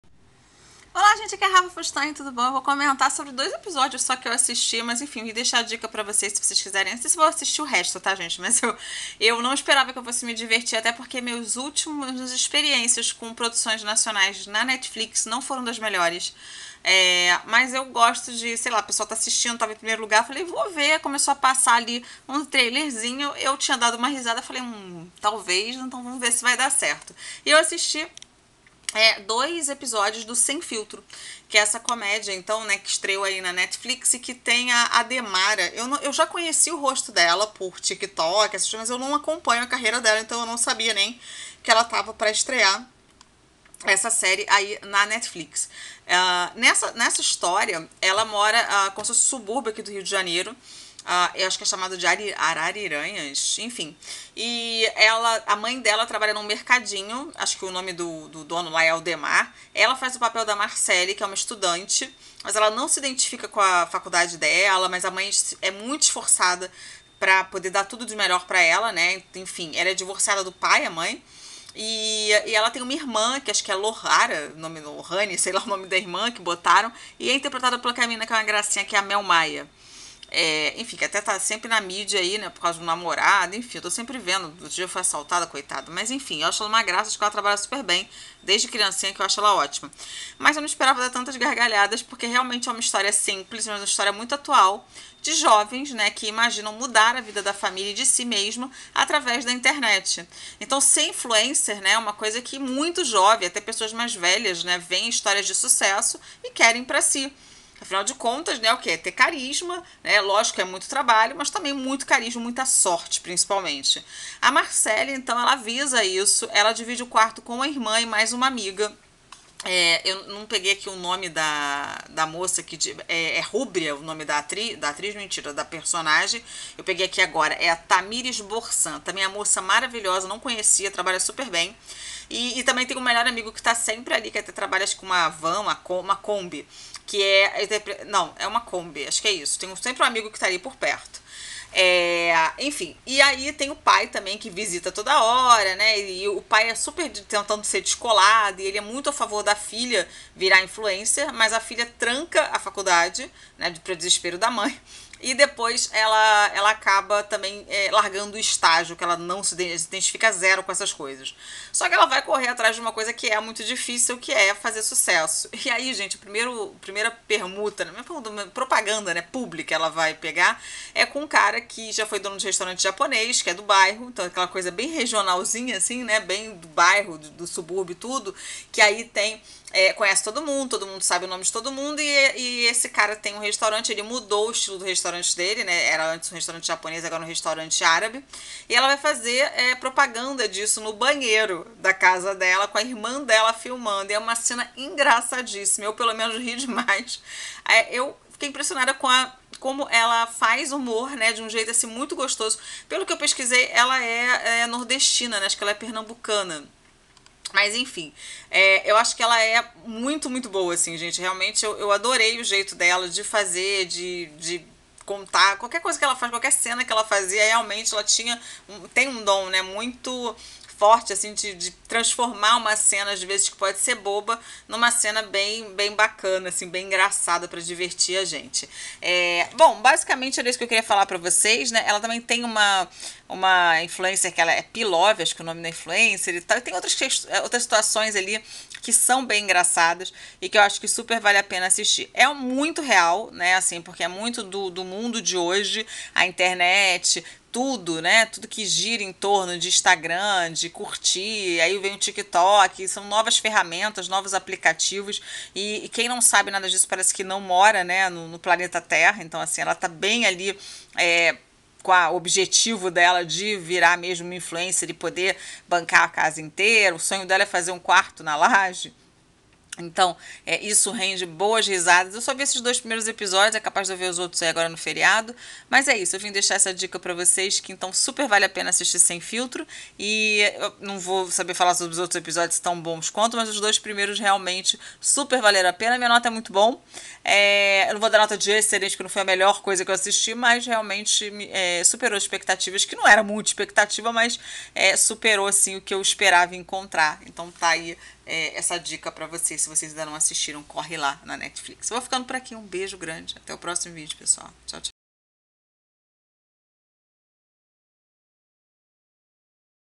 Sem Filtro 🎬 fui surpreendida positivamente por essa série nacional
Sem Filtro – 1ª Temporada (Original Netflix)
Título Original: Sem Filtro
Duração: 340 minutos
Ano produção: 2022
Estreia: 15 de fevereiro de 2023
Distribuidora: Netflix
Dirigido por: Cris D’Amato
Orçamento:
Classificação: 12 anos
Gênero: Comédia
Países de Origem: Brasil
Sinopse:
Marcely não aguenta mais o curso da faculdade e decide largar tudo para realizar seu novo sonho: ser uma influencer. Só que a vida online é mais difícil do que parece…

Elenco:
Ademara, Flávia Reis, Orã Figueiredo, Mel Maia, Thamirys Borsan, Pedro Ottoni, Maicon Rodrigues

Curiosidades:
A série foi criada por João Paulo Horta.
A direção geral da série é de Cris D’Amato (Pai em Dobro).
Alguns nomes presentes no elenco são Flávia Reis (Não Vamos Pagar Nada), Mel Maia (Tudo por um Pop Star) e Pedro Ottoni (Pai em Dobro)